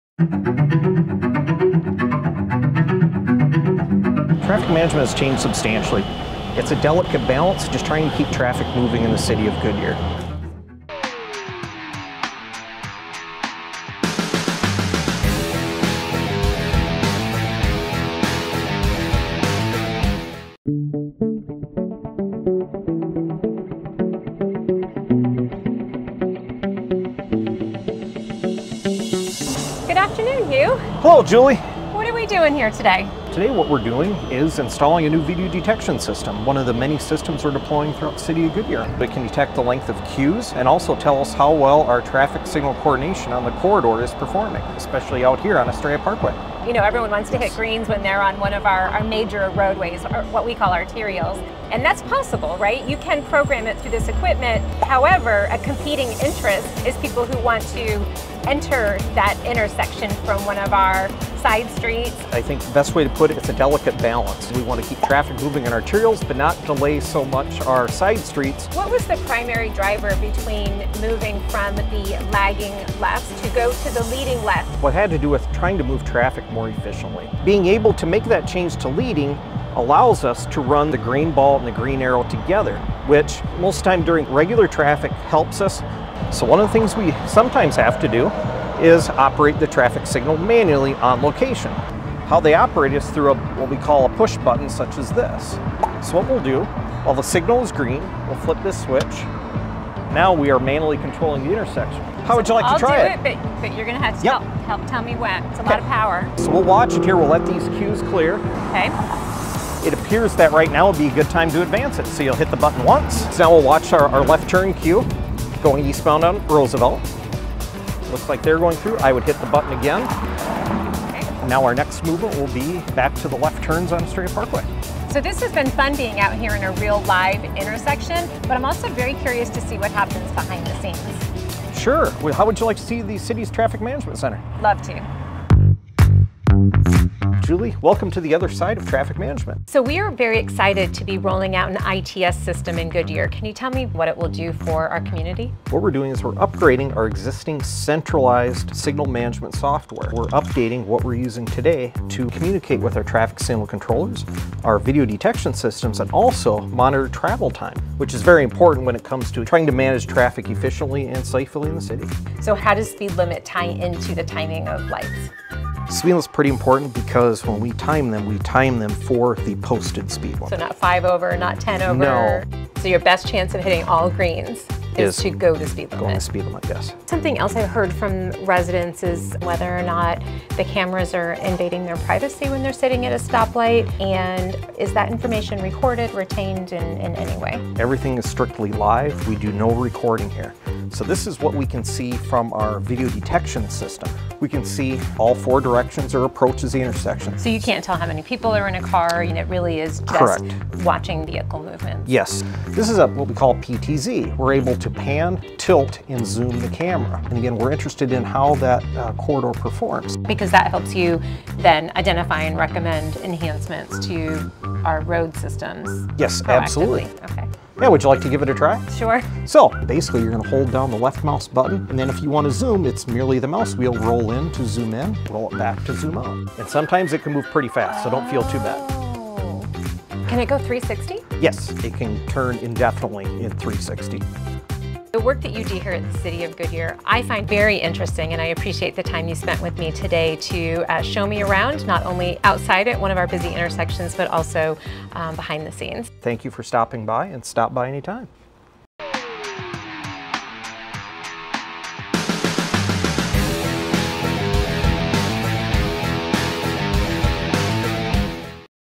On the Job with Julie: Traffic in Goodyear
When it comes to traffic in Goodyear, change is the only constant. Goodyear City Manager Julie Karins spent some time with City Traffic Engineer Hugh Bigalk to learn about how traffic lights work in the city, and why Goodyear switched from lagging to leading left turn arrows.